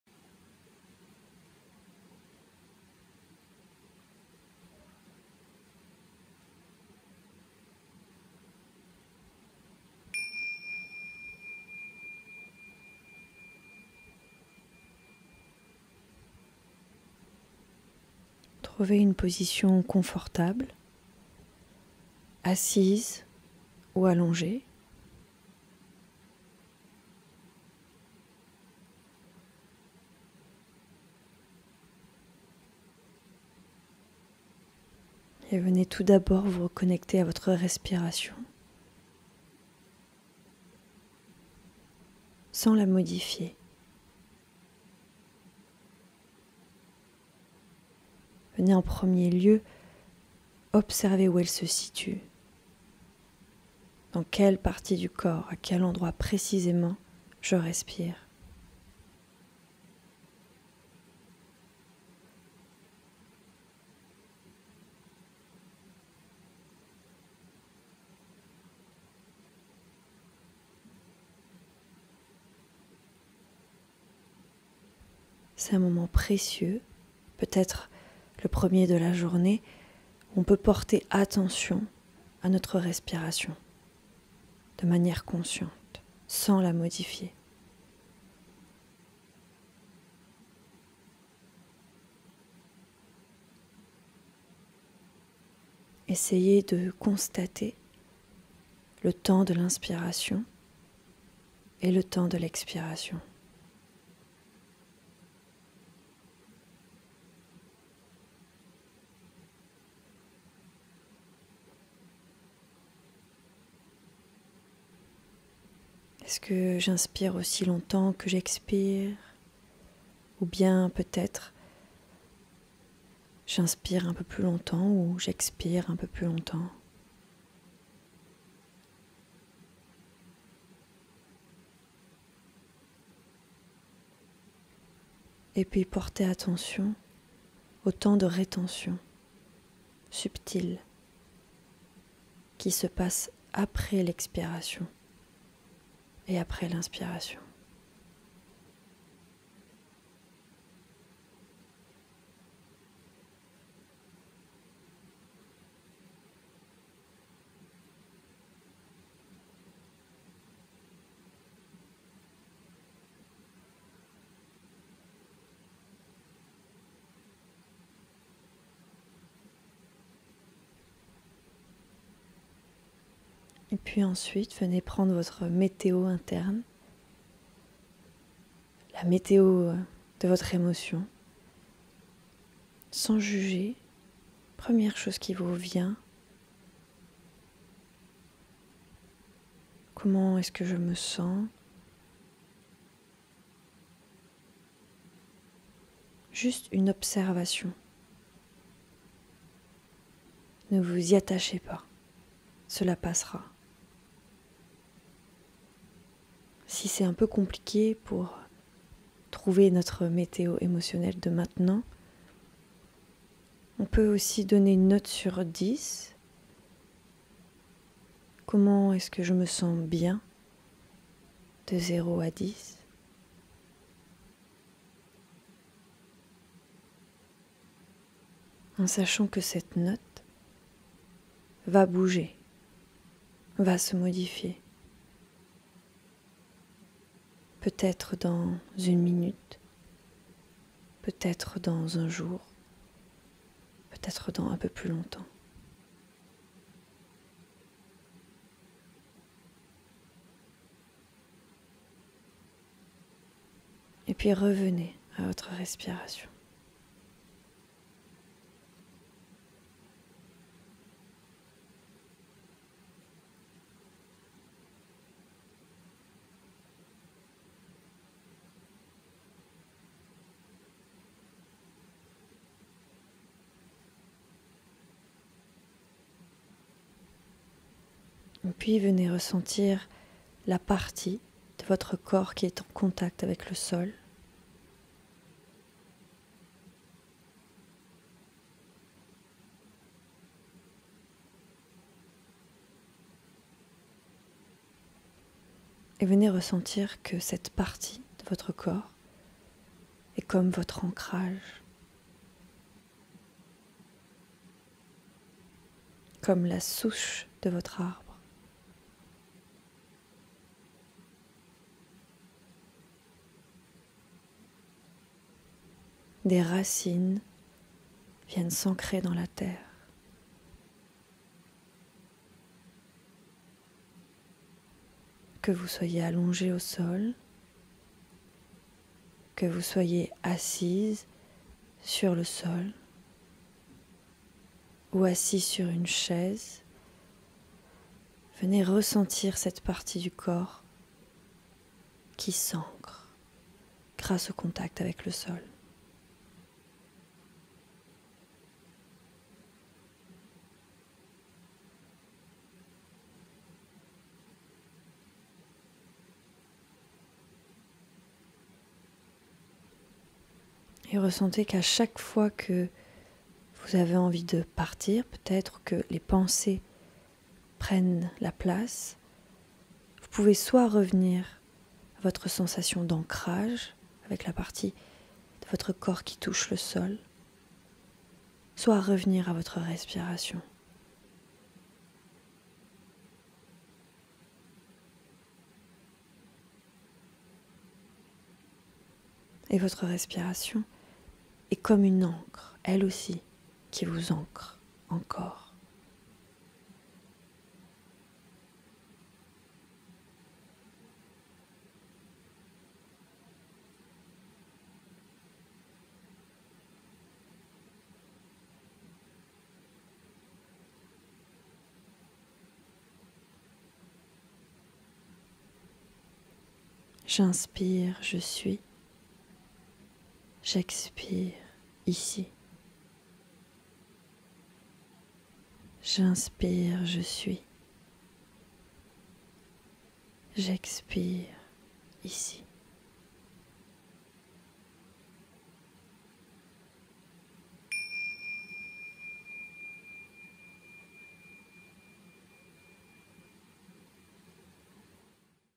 MÉDITATION GUIDÉE | 7min pour s'ancrer
Yoyoyo les yogis !

Prenez le temps de vous assoir au sol, sur une chaise les pieds à plat, ou bien de vous allonger.
N'hésitez pas à mettre un casque sur les oreilles, à fermer les yeux et laissez vous guider pendant 7 minutes.
L'ancrage est la meilleure façon de revenir à l'instant présent, ici et maintenant.
Être à l'écoute de soi, de sa météo interne, et de sa respiration.

Bienvenue dans le calme

💬 Partagez vos progrès et posez vos questions dans les commentaires ci-dessous.